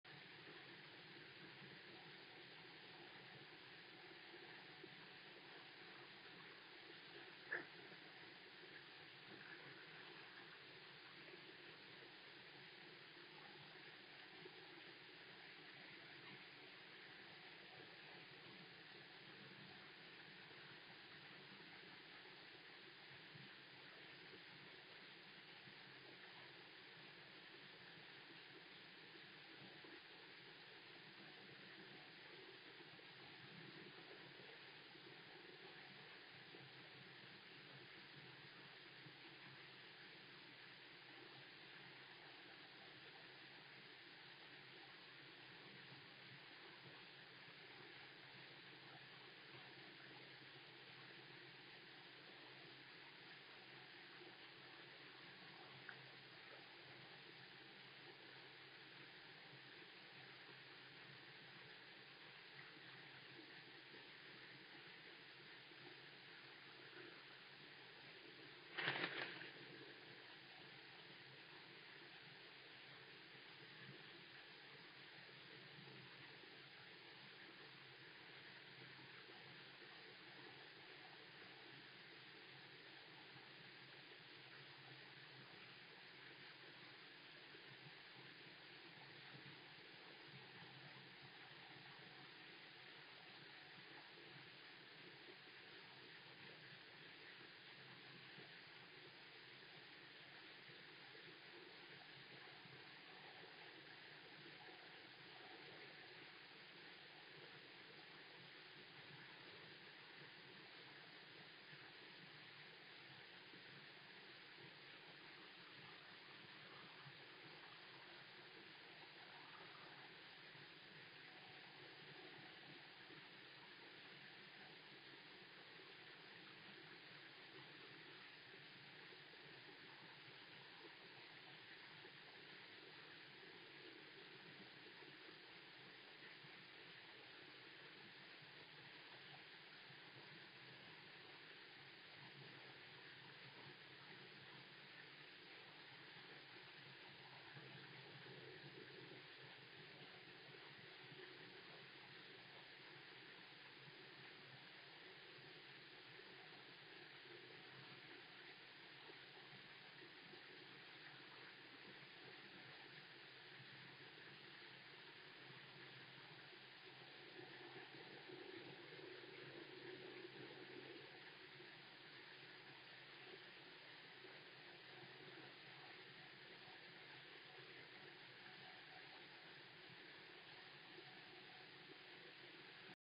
Ville de Sarralbe Télé Cigognes Webcam Live Sarralbe, Moselle Google Chrome 2024 06 09 21 15 3
SARRALBE en MOSELLE , CETTE NUIT LA FAMILLE EST AU NID , LA FEMELLE S ' OCCUPE DU PLUS GRAND  ET LE MÂLE  SURVEILLE LE NID .